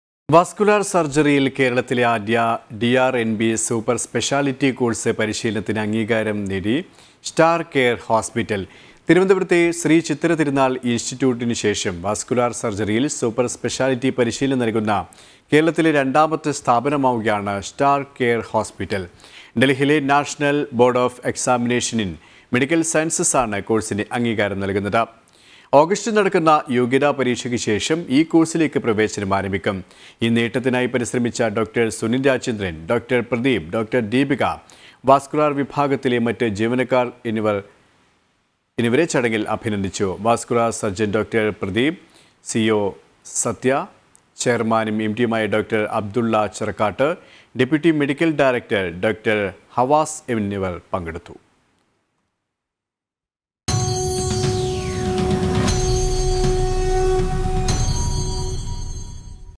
കേരളത്തിലെ ആദ്യ DRNBസൂപ്പർ സ്പെഷ്യാലിറ്റി കോഴ്സ് പരിശീലനത്തിനു അംഗീകാരം നേടി സ്റ്റാർ കെയർ ഹോസ്പിറ്റൽ
വാസ്കുലാർ സർജറിയിൽ കേരളത്തിലെ ആദ്യ DRNBസൂപ്പർ സ്പെഷ്യാലിറ്റി കോഴ്സ് പരിശീലനത്തിനു അംഗീകാരം നേടി സ്റ്റാർ കെയർ ഹോസ്പിറ്റൽ